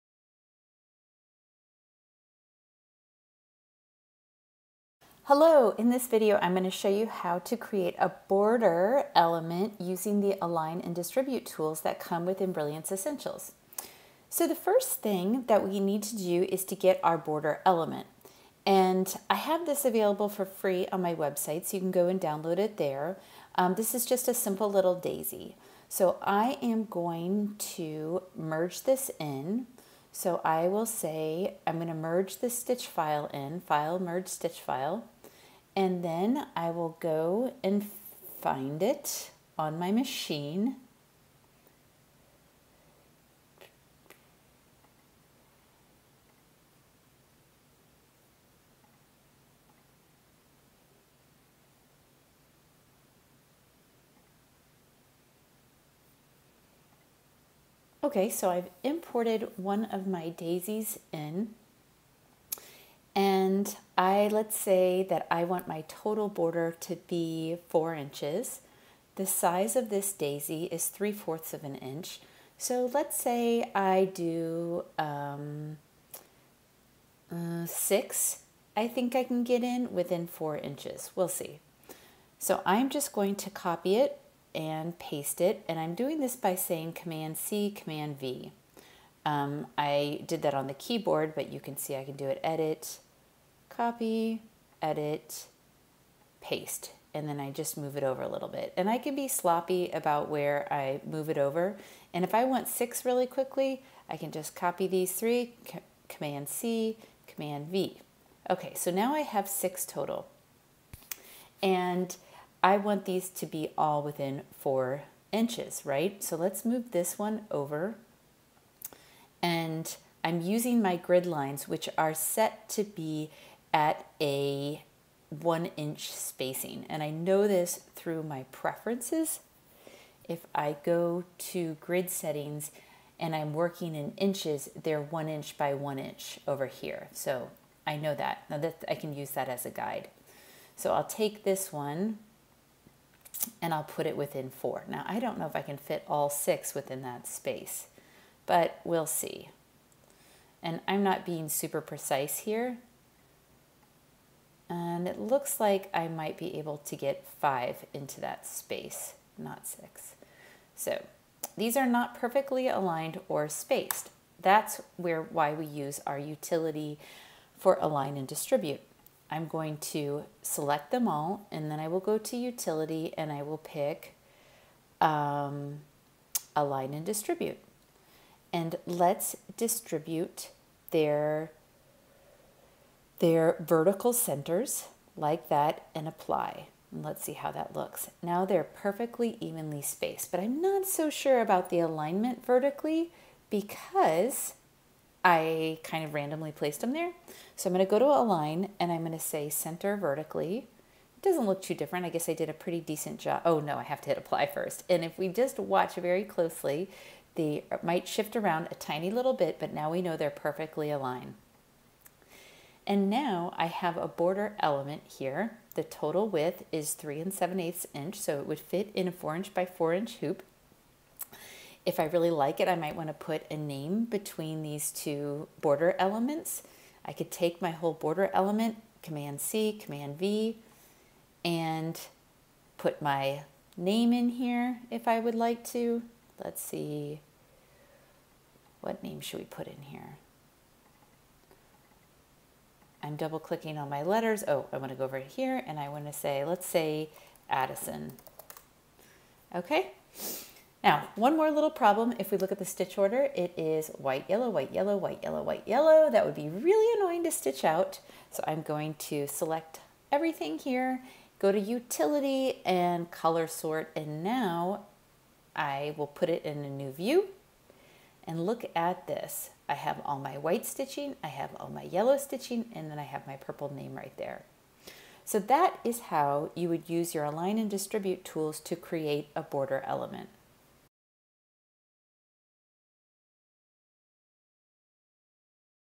How to create a border design from individual embroidery designs in Embrilliance Essentials
Embrilliance Essentials allows you to do more with your embroidery designs - like create a border! It's simple.  All you have to do is merge one instance of the design into your page and then copy and paste, use your align + distribute tool and color sort.

How to use Embrilliance Essentials?

Embrilliance Essentials vs. Embrilliance Enthusiast

Video highlights:
0:00 welcome
0:18 merge in daisy design
1:23 duplicating design
2:46 using Align and Distribute tool
3:46 duplicating border
4:01 adding a name
4:15 color sort